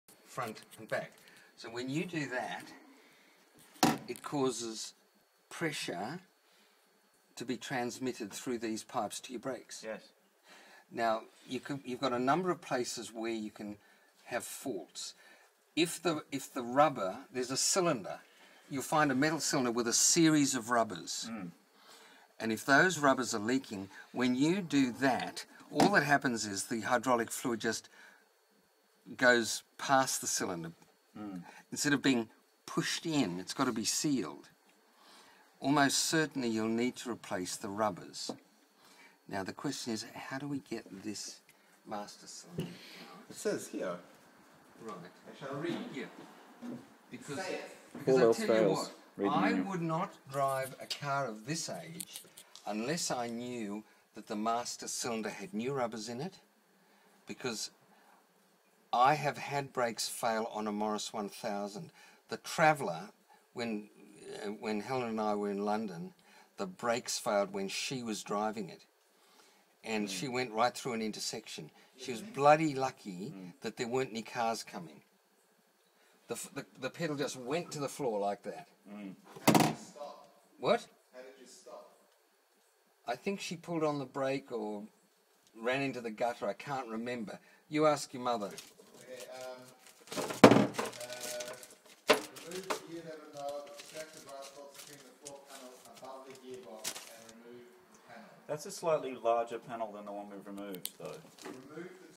Investigating the brake master cylinder
Paul describes how the master cylinder works and recalls when the brakes failed in his Morris minor...